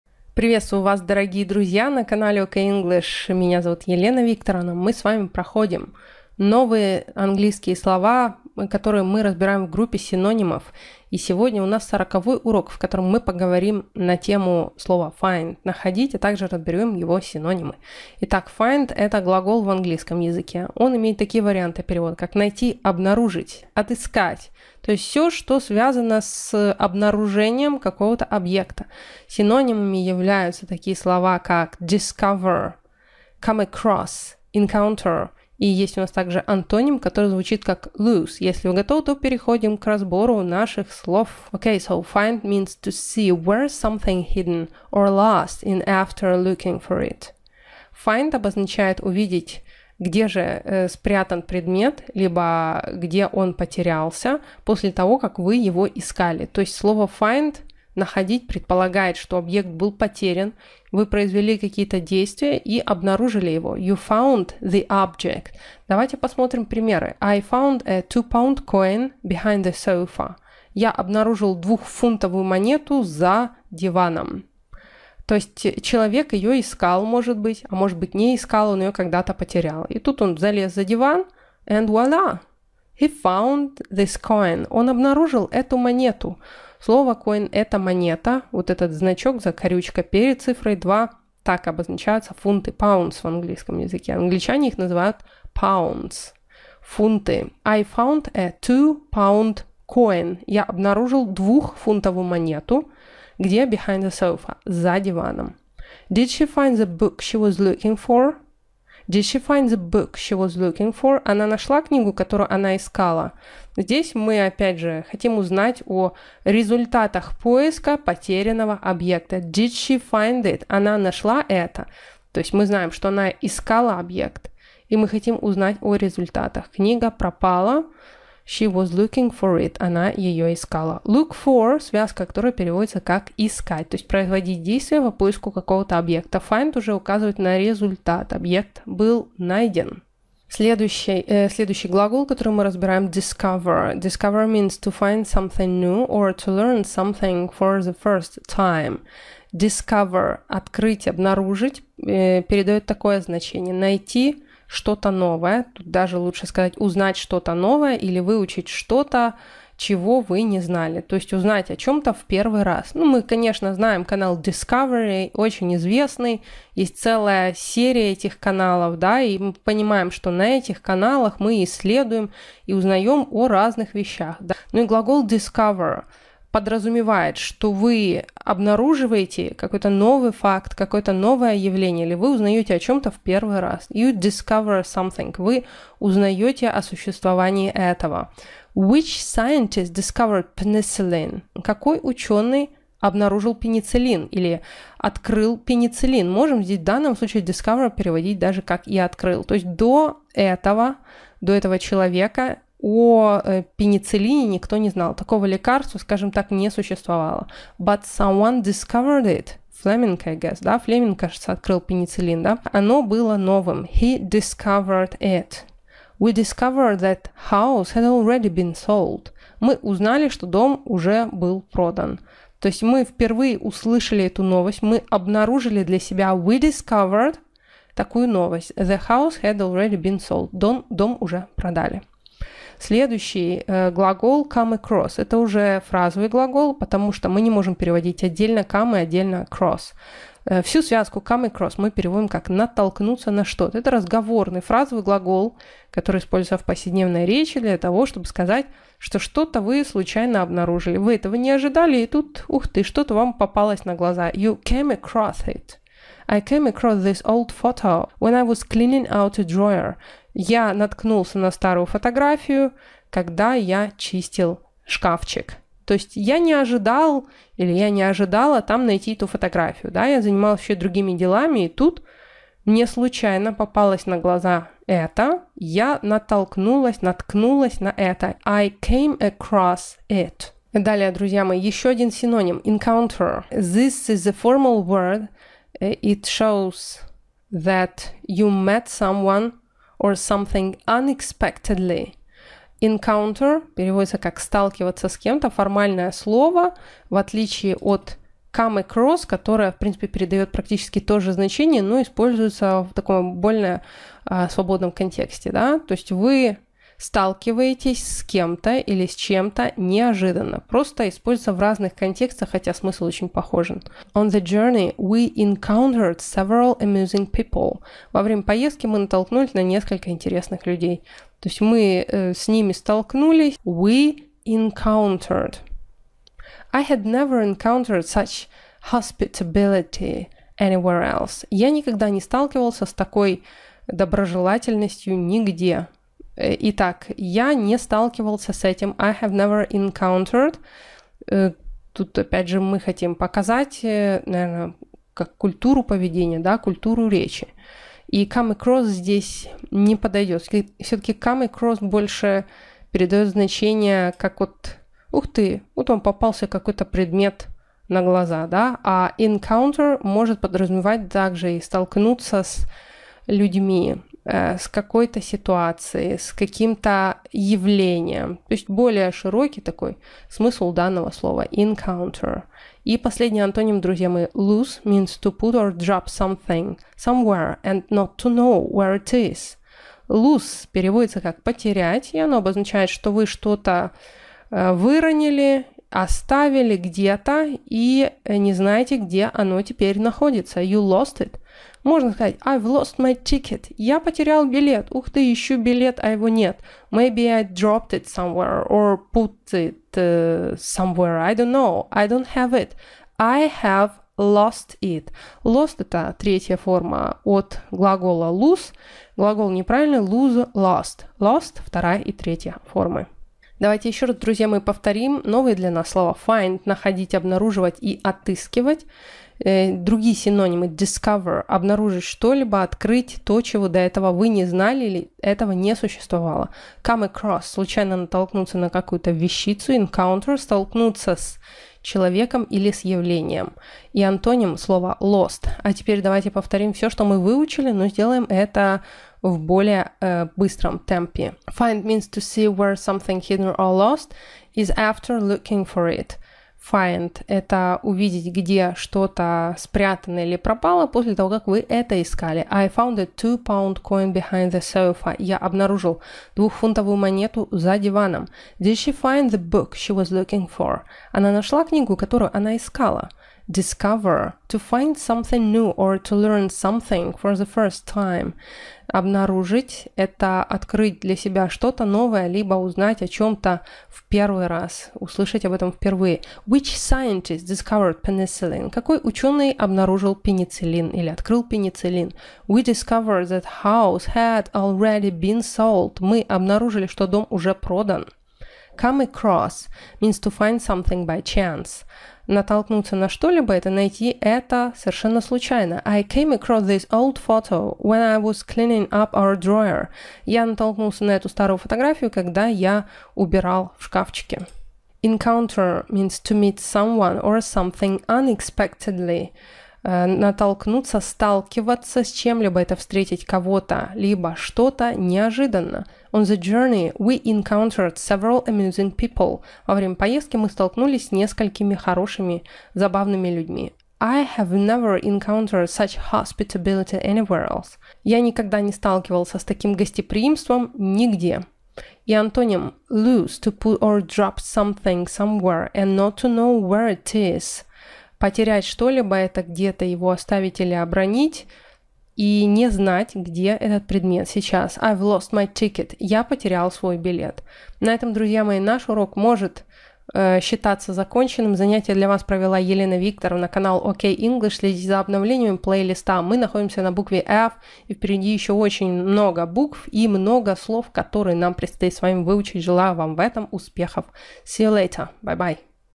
#40 Find - Находить 📘 Intermediate vocabulary of synonyms | OK English
Книга:
Basic synonyms (pre-intermediate), издательство Bloomsbury, 2004 год
Синонимы: find, discover, come across, encounter
Антоним: lose